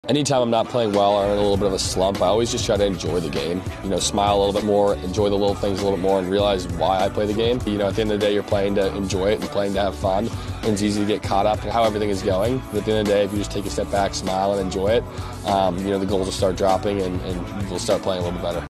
In a Slump? FCL Athlete Tucker Dordevic Gives a Quick Mental Tip for Lacrosse Players!
Tucker Dordevic is in his graduate transfer year at Georgetown. Georgetown is a big time NCAA title contender in 2023. Tucker grew up in Oregon and was recently announced as a First Class Lacrosse Athlete!